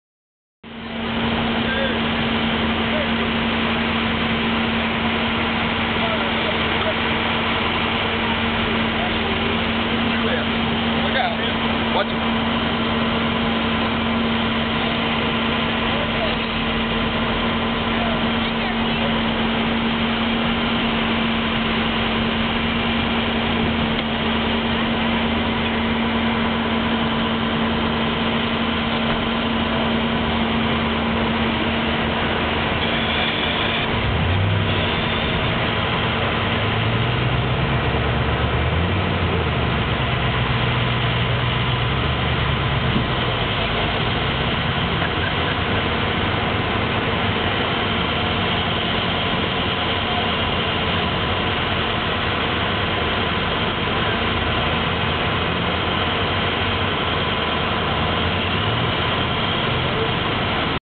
Rollover On Calhoun City Square
Ella Woods was not injured when she crashed on the Calhoun City Square Friday, July 20.